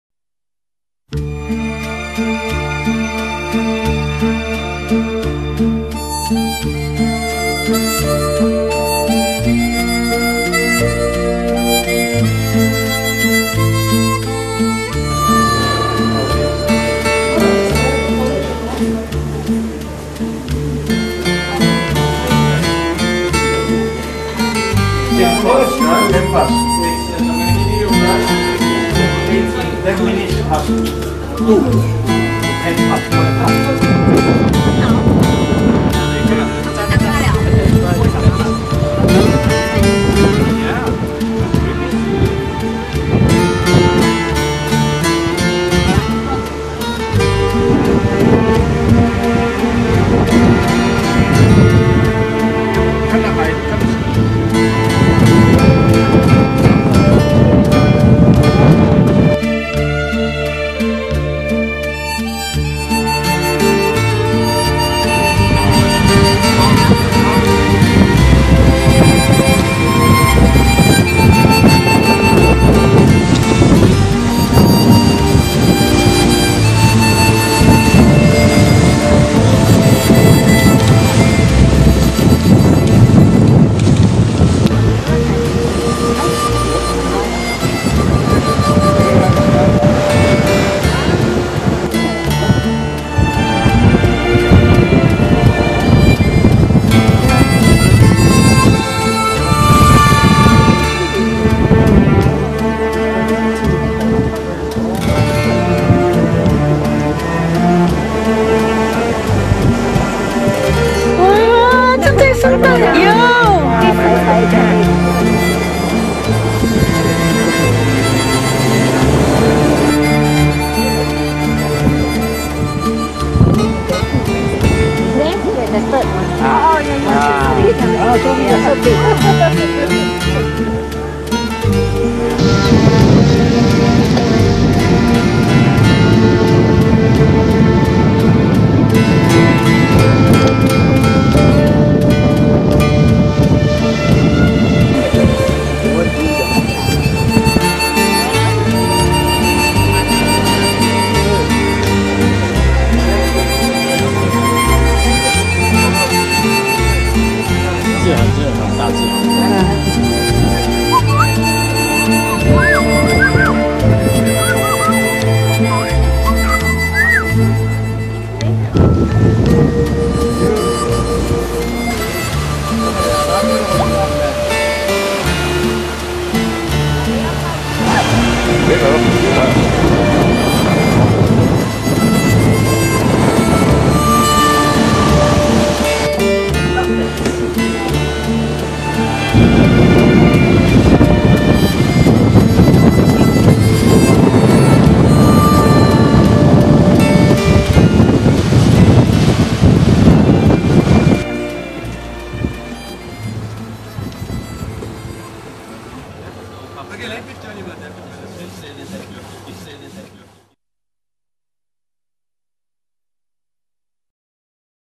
South Africa-Cape Town-Boulders Beach-African penguins
Nestled in a sheltered cove between Simon's Town and Cape Point, Boulders Beach has become world famous for its thriving colony of South African Penguins and magnificent wind sheltered, safe beaches.